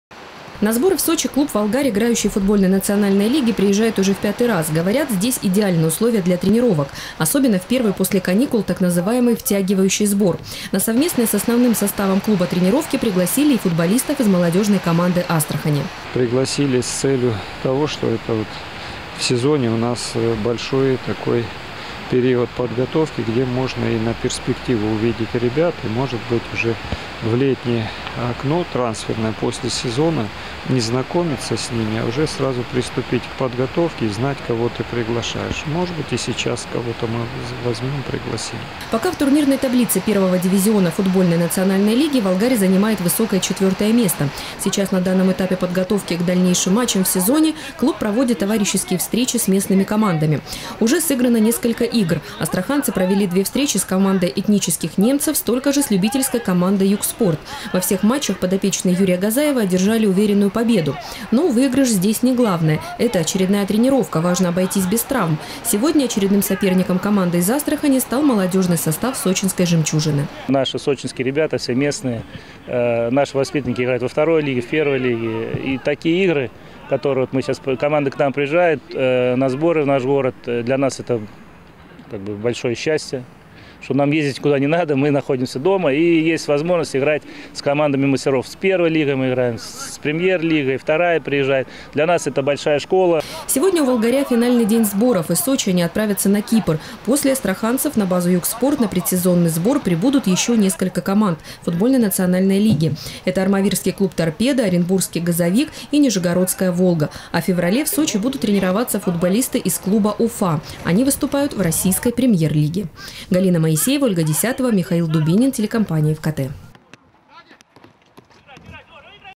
В Сочи сборы завершает футбольный клуб «Волгарь». Новости Сочи Эфкате
На базе «Юг-спорт» они будут тренироваться до завтра. Сегодня в финальный день сборов астраханцы сыграли товарищеский матч с молодёжной командой «Жемчужина».
Сочи. Новости.
Телеканал Эфкате.